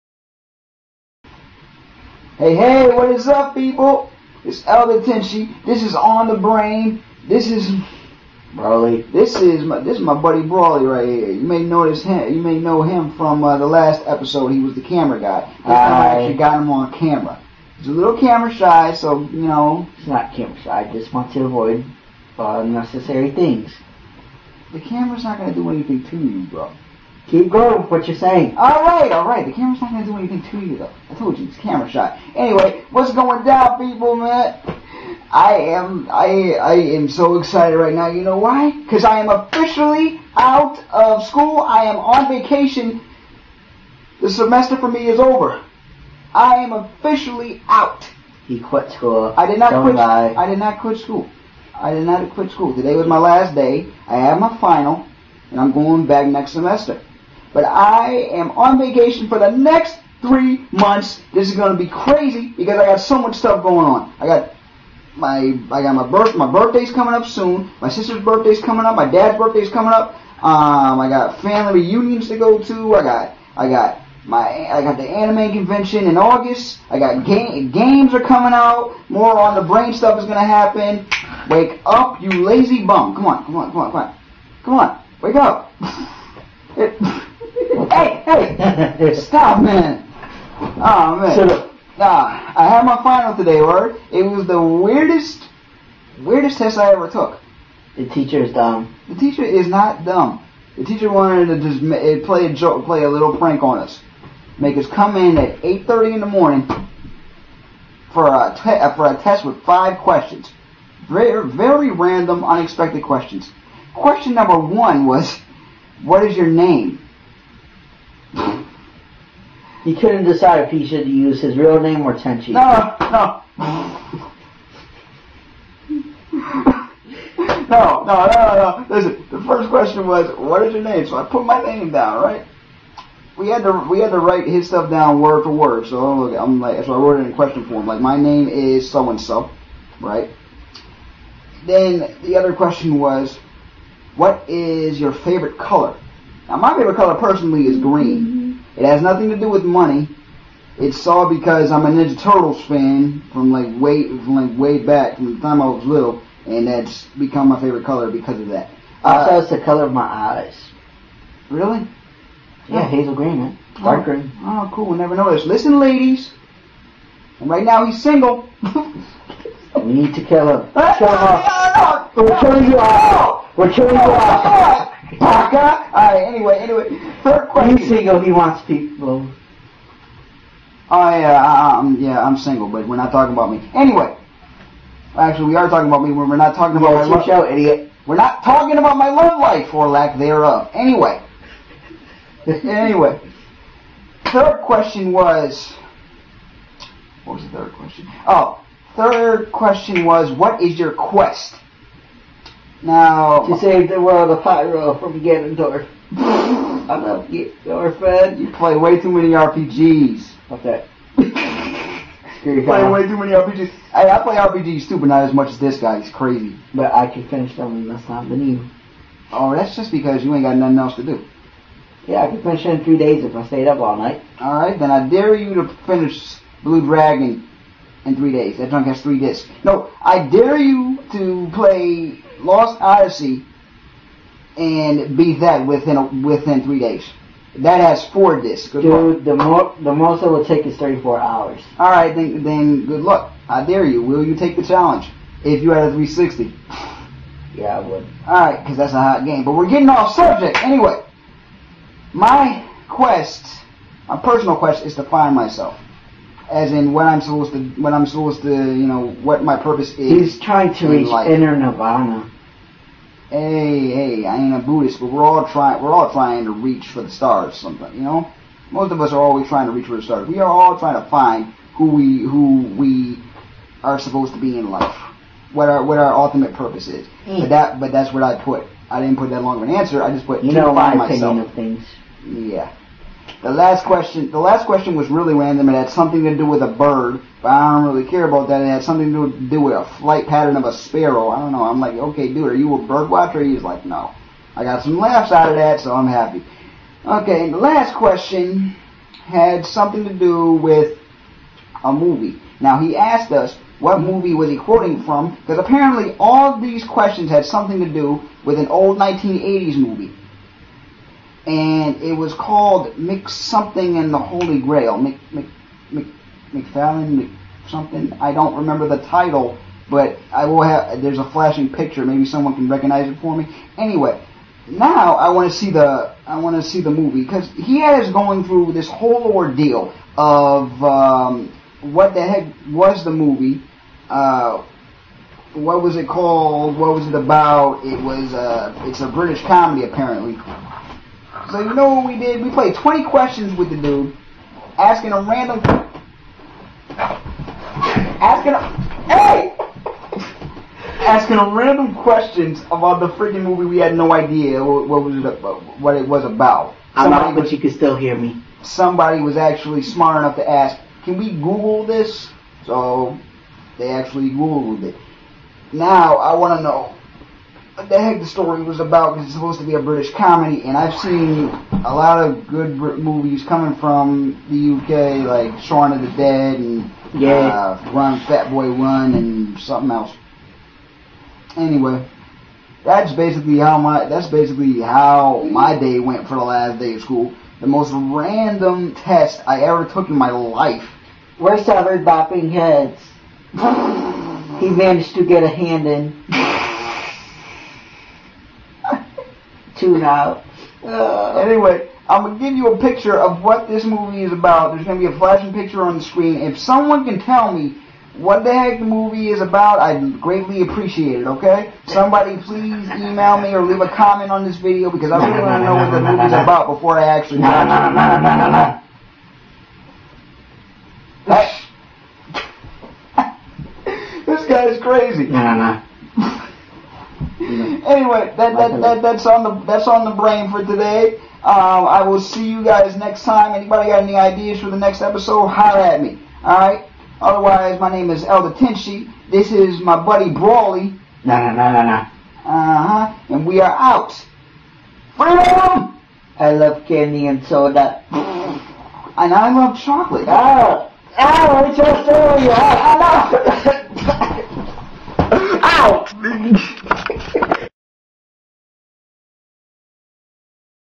On The Brain episode 6 (The Random exam)
Today was the last day of this semester of college for me and the day that I had to take the most random test of my life! Watch this episode of OTB and you'll see what I mean. P.S. Please rate and comment.